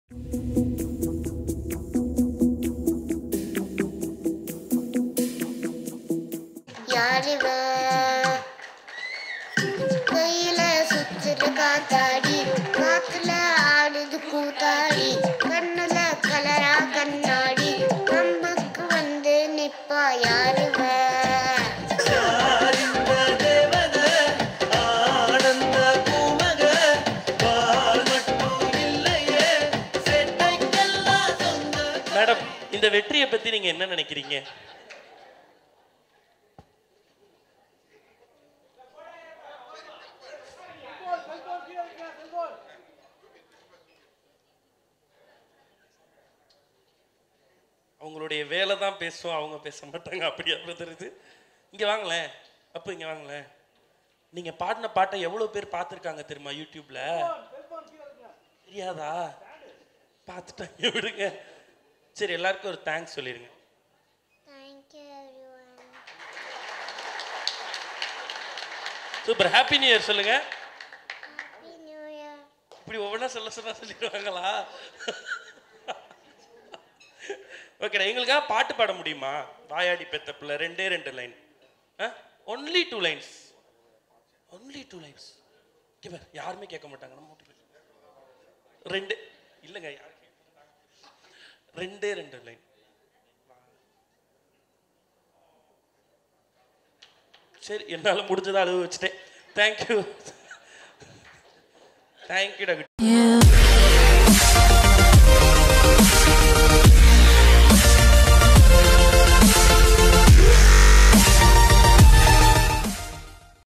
Sivakarthikeyan Daughter Aradhana First Ever Cute Speech at Kanaa Success Meet | Aaradhana
ivakarthikeyan Daughter Aradhana First Ever Cute Speech at Kanaa Success Meet | Aaradhana

Like us @